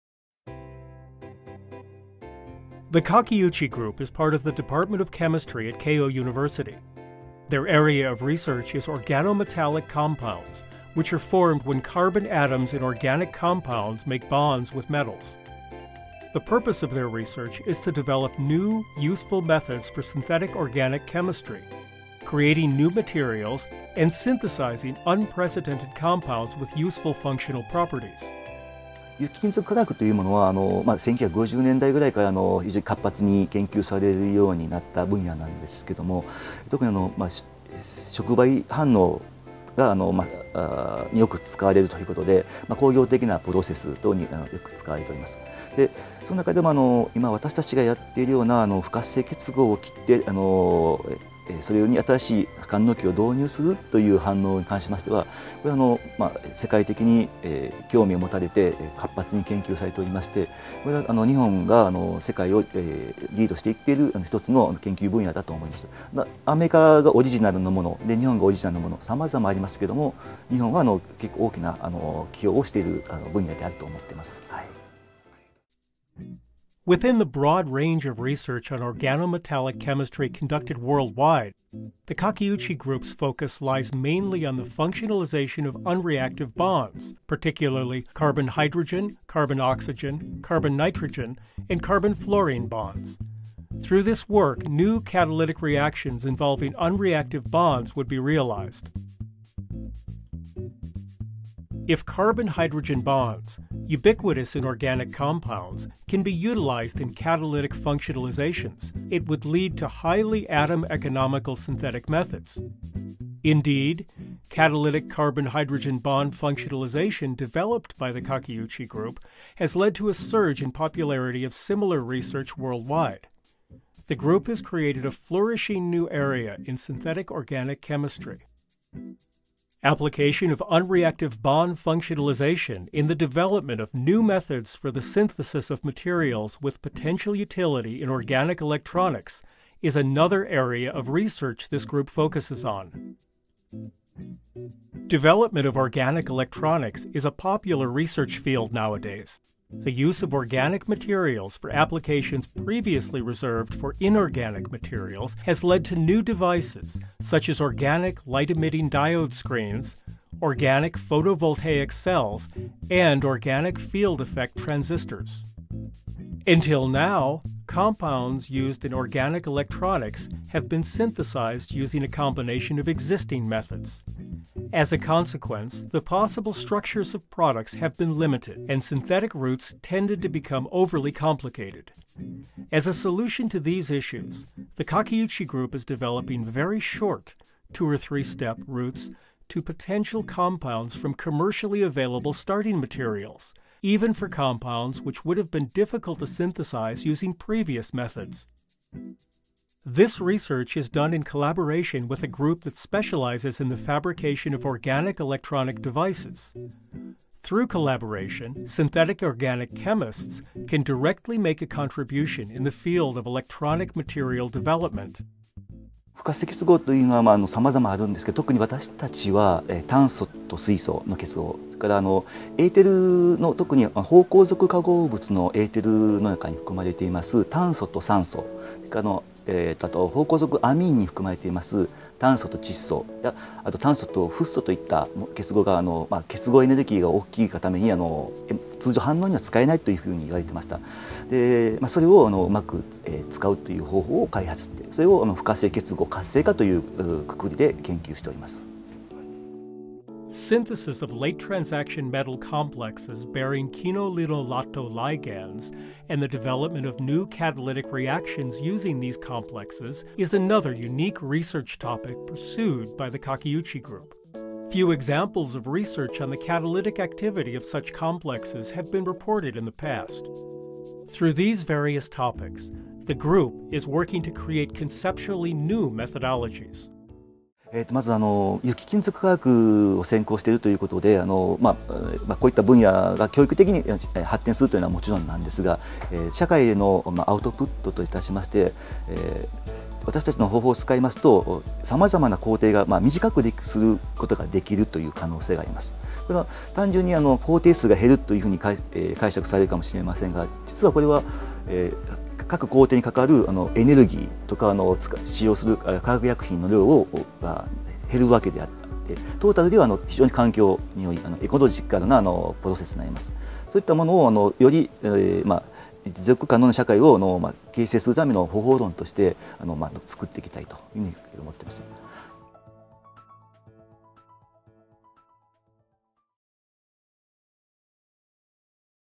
Toward an ecological, sustainable society by utilizing organometallic compounds
The Kakiuchi Group is part of the Department of Chemistry at Keio University. Their area of research is in organometallic compounds, which are formed when carbon atoms in organic compounds make bonds with metals. The purpose of their research is to develop new, useful methods for synthetic organic chemistry, creating new materials, and synthesizing unprecedented compounds with useful functional properties.

Q. Extensive researches on organometallic chemistry have taken place since the 1950s. Organometallic compounds are often used in industrial catalytic processes. Our research on introduction of functional groups via the cleavage of unreactive bonds attracts worldwide attention from others in the field. I believe Japan leads the world in this field. Surely, some aspects originated in the US, while others did in Japan, but overall, I believe Japan's contribution is overwhelming.

Within the broad range of research on organometallic chemistry conducted worldwide, the Kakiuchi Group's focus lies mainly on the functionalization of unreactive bonds, particularly carbon-hydrogen, carbon-oxygen, carbon-nitrogen and carbon-fluorine bonds. Through this work, new catalytic reactions involving unreactive bonds would be realized.

If carbon-hydrogen bonds, ubiquitous in organic compounds, can be utilized in catalytic functionalizations, it would lead to highly atom-economical synthetic methods. Indeed, catalytic carbon-hydrogen bond functionalization developed by the Kakiuchi Group has led to a surge in popularity of similar researches worldwide. The Group has created a flourishing new area in synthetic organic chemistry.

Application of unreactive bond functionalization in the development of new methods for the synthesis of materials with potential utility in organic electronics is another area of research this Group focuses on.

Development of organic electronics is a popular research field nowadays. The use of organic materials for applications previously reserved for inorganic materials has led to new devices such as Organic Light-Emitting Diode Screens, Organic Photovoltaic Cells and Organic Field Effect Transistors.

Until now, compounds used in organic electronics have now been synthesized using a combination of existing methods. As a consequence, the possible structures of products have been limited, and synthetic routes tended to become overly complicated. As a solution to these issues, the Kakiuchi Group is developing very short, (two to three step) routes to potential compounds from commercially available starting materials, even for compounds which would have been difficult to synthesize using previous methods.

This research is done in collaboration with a group that specializes in the fabrication of organic electronic devices. Through collaboration, synthetic organic chemists can directly make a contribution in the field of electronic material development.

Q. There are many known unreactive bonds, but out of those, we focused especially on the carbon-hydrogen bond. Carbon-Oxygen bonds in aromatic ethers, carbon-nitrogen bonds in aromatic amines, and carbon-fluorine bonds are other unreactive bonds we look into. Because of their high bond dissociation energies, traditionally these unreactive bonds would not have been suitable reaction points for functionalizations. Our research aims at using these bonds effectively, and investigation on how to activate these bonds.

Synthesis of late-transition-metal complexes bearing quinolinolato ligands and the development of new catalytic reactions using these complexes is another unique research topic pursued by the Kakiuchi Group. Few examples of research on the catalytic activity of such complexes have been reported in the past. Through these various topics, the group is working to create conceptually new methodologies.

Q. Through our research in organometallic chemistry, not only can we contribute to scientific advancement, but also to the society, as our methods can potentially streamline various existing processes. While reducing a few reaction steps may sound like something minor, in reality, it has a huge effect on reducing the amount of energy and chemicals required. Streamlining processes make them more environmentally benign. We would like to create such options for realization of a more sustainable society.